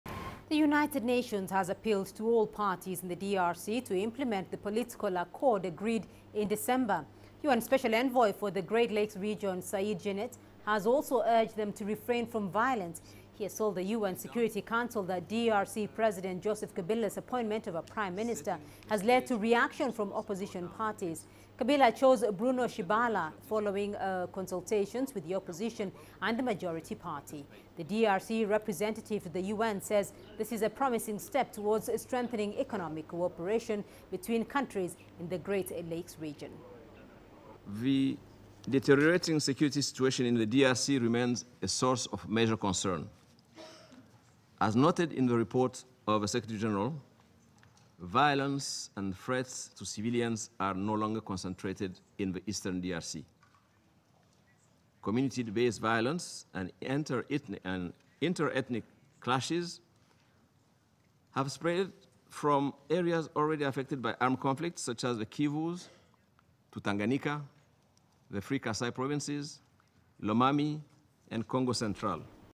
UN urges DRC to implement December peace accord
The United Nations has appealed to all parties in the DRC to implement the political accord agreed in December. UN Special Envoy for the Great Lakes region, Said Djinnit, has also urged them to refrain from violence. Djinnit has told the UN Security Council that DRC President Joseph Kabila's appointment of a Prime Minister has led to reaction from opposition groups. Kabila chose Bruno Tshibala following consultations with the opposition and the majority party. The DRC Representative to the UN says this is a promising step towards strengthening economic cooperation between countries in the Great Lakes region.